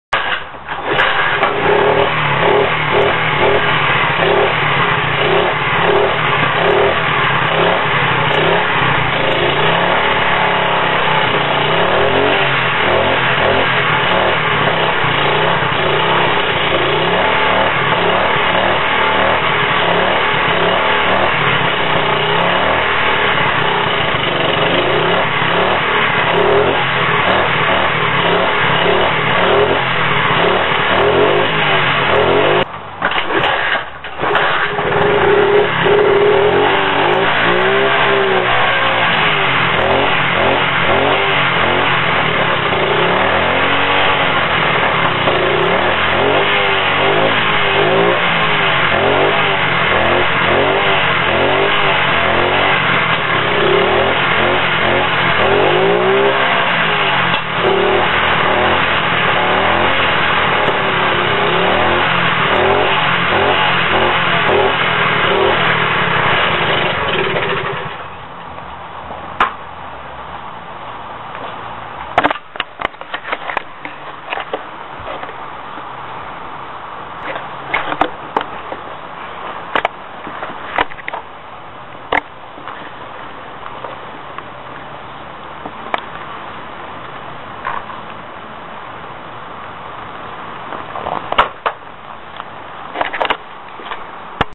how to:cold start a dirtbike
me starting my new dirt bike cold start needs carb tuning